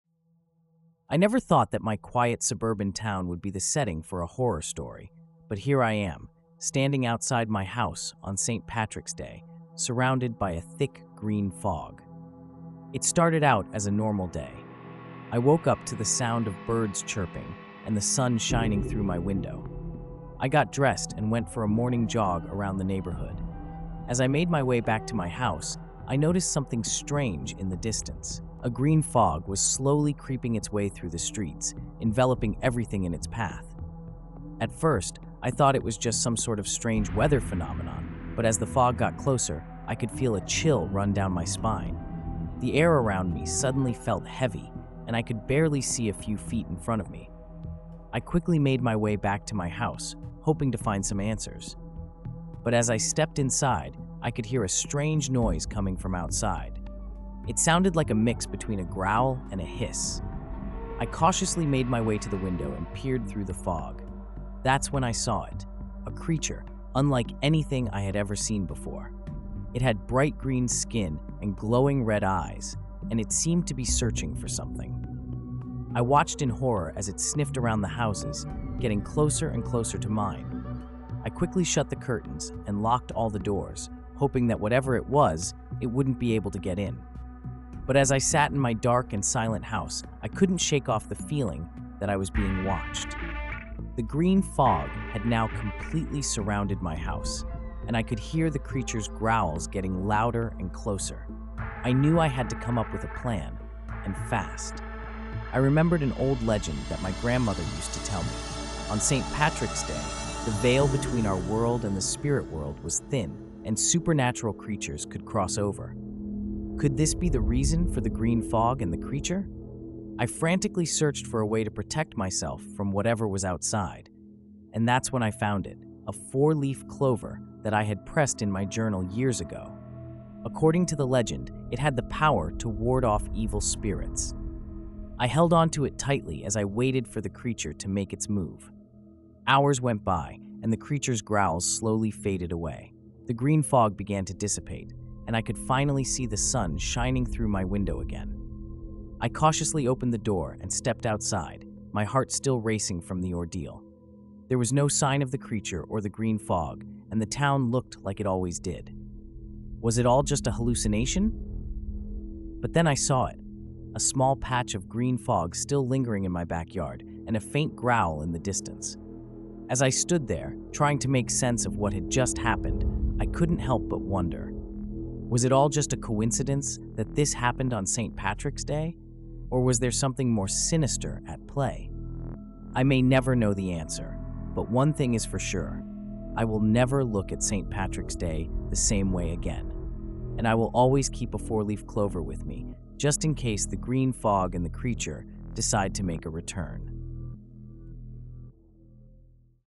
The Green Fog - A Saint Patrick's Day Horror Story
Saint Patrick's Day turns into a horror story when a green fog and a mysterious creature invade a quiet suburban town

Elements of this video are created with generative AI